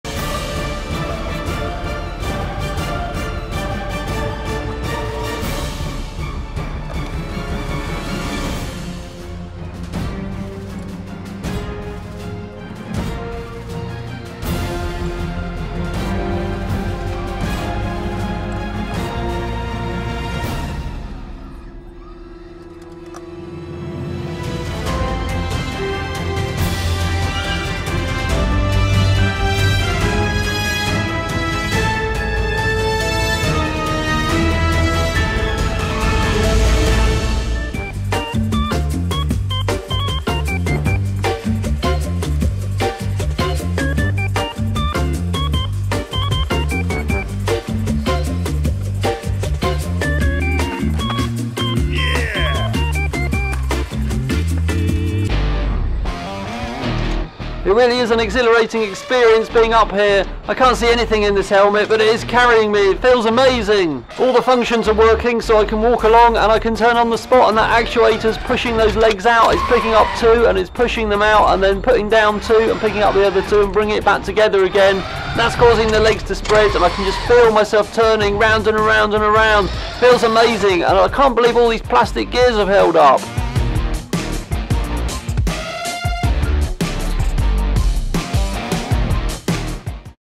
I built a real Star Wars AT-AT
I built a a Star Wars AT-AT big enough that I can ride on it. I used lots of large 3D printed parts and aluminium extrusion to build the Star Wars AT-AT. It's really strong and can dead-lift both me and itself. It can turn and walk along with me on top.